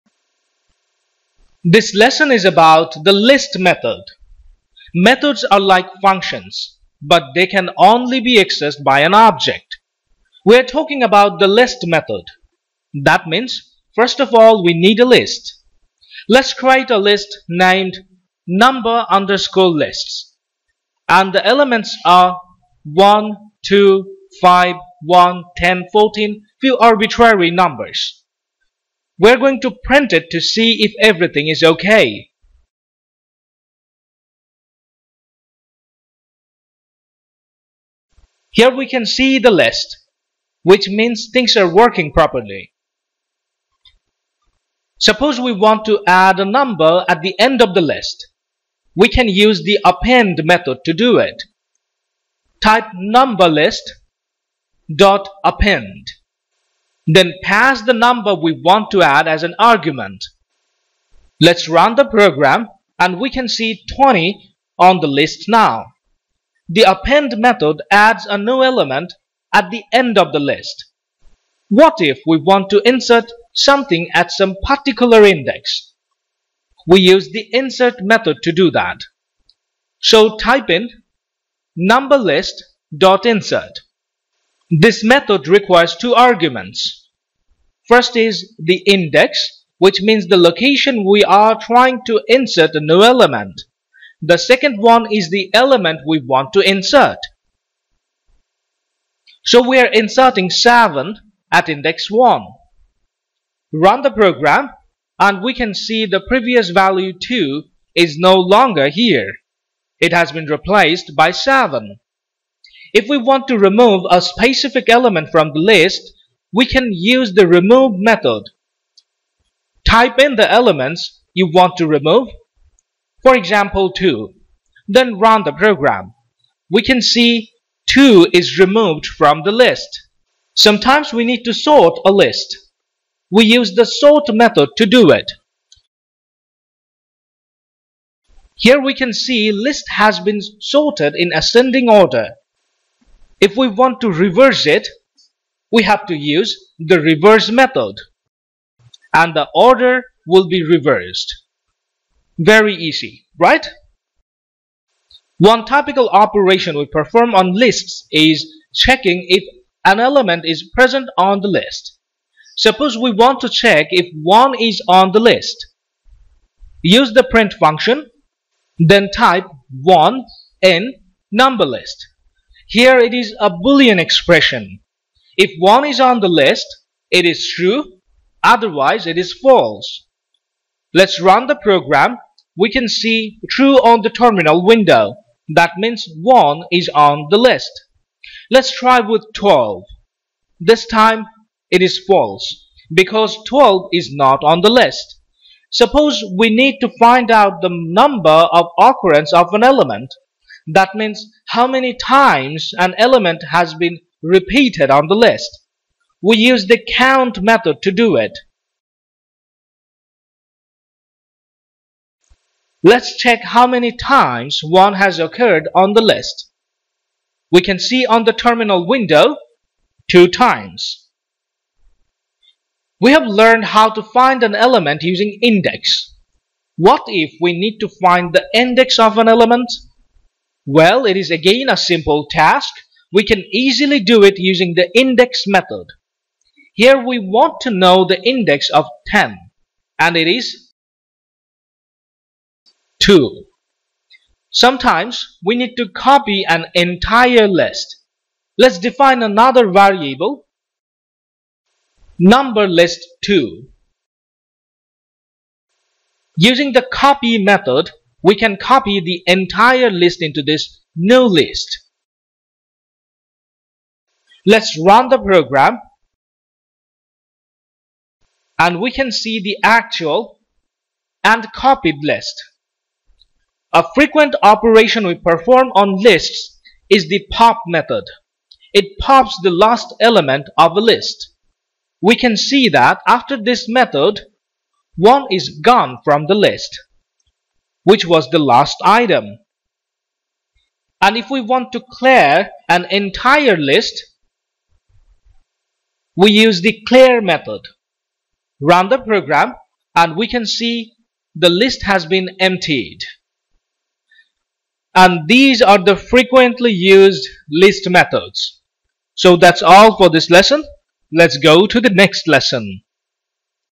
Python List Method
Lists are handy data structure in Python. When we work in Machine Learning or Data Science, lists are unavoidable. We need to learn frequently used list methods to use python lists effectively. In this lesson, we are going to learn the standard list methods of Python.

Methods are like functions. However, they are different. Because it can be called using the designated object only. For example, if we want to use any list methods, we have to create a list object first. The good news is that any list in Python is an object. So, whenever we declare a list, we declare a list object. And we can access the methods defined for the list using the ‘dot’ operator.

And in this lesson, we will learn how to create a list, how to access the methods using the dot operator, and the standard methods.